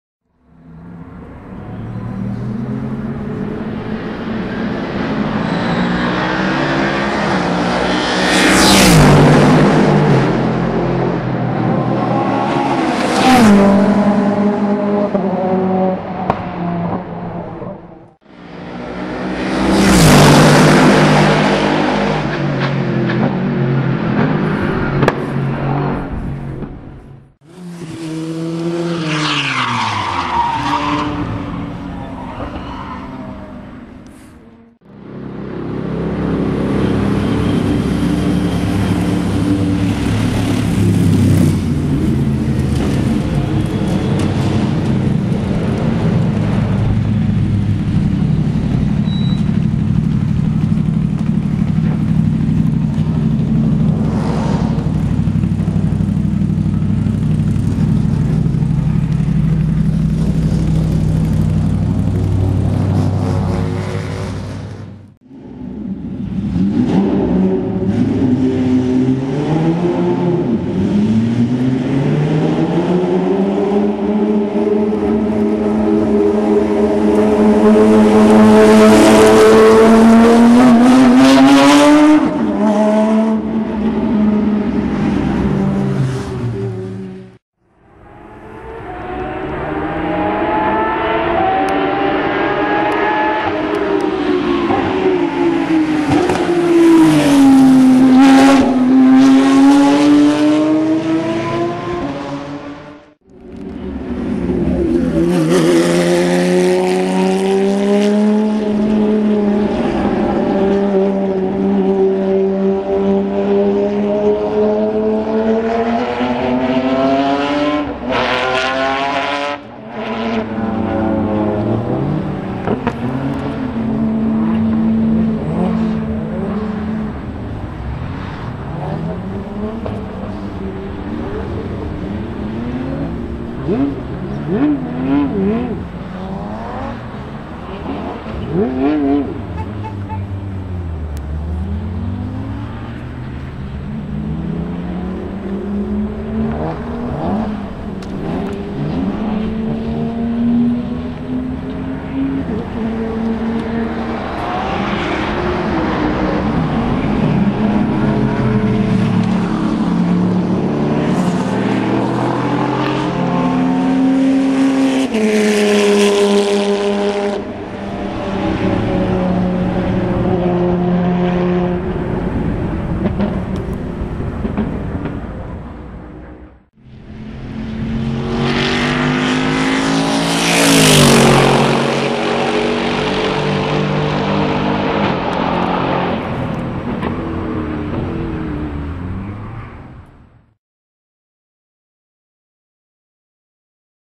Mercedes-Benz SLS AMG GT3, Porsche supercup (testday circuit zolder)
international testday at circuit zolder on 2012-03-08
Some of the cars in this video:
Formule Ford, Mercedes-Benz SLS AMG GT3, Porsche 997 GT3 supercup, Norma M20F, Renault Clio2 cup